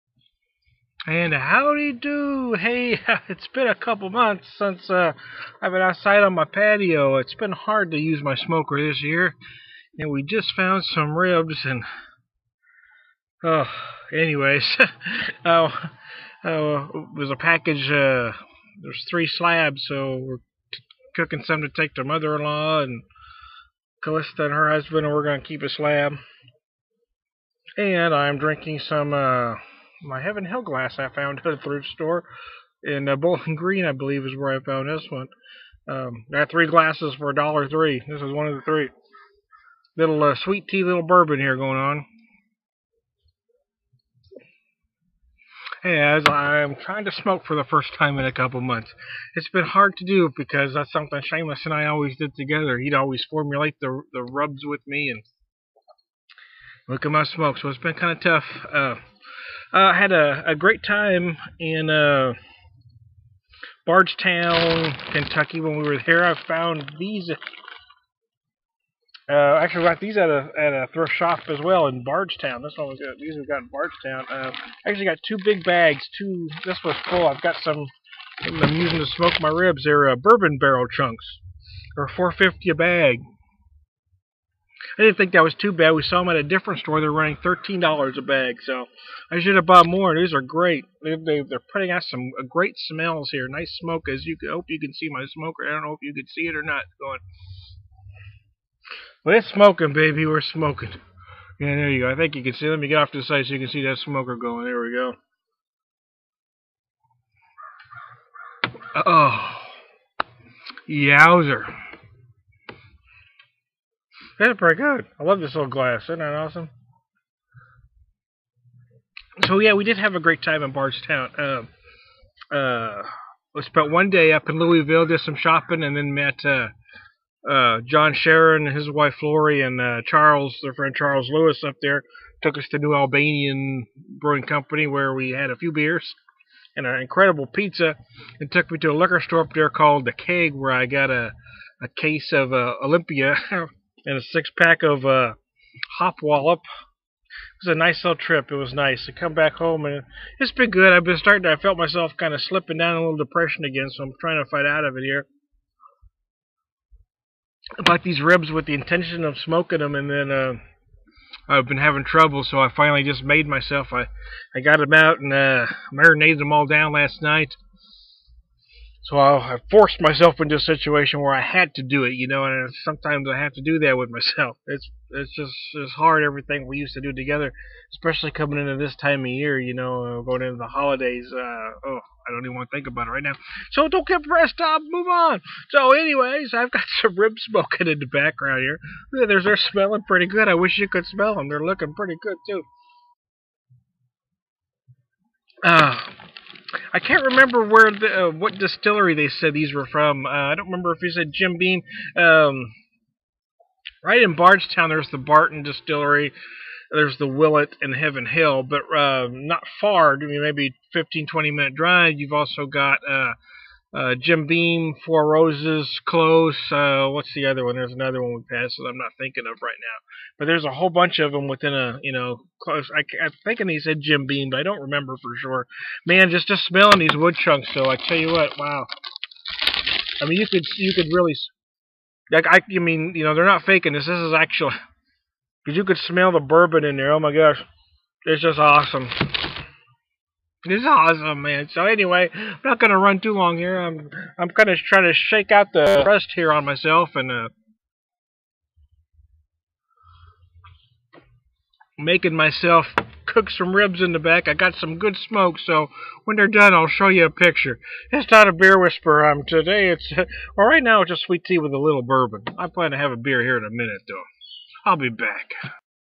Drinkin' With The Beer Whisperer:Talking About Ribs, Seamus And Sweet Tea and Bourbon!
First time smoking ribs in a while and remembering Seamus.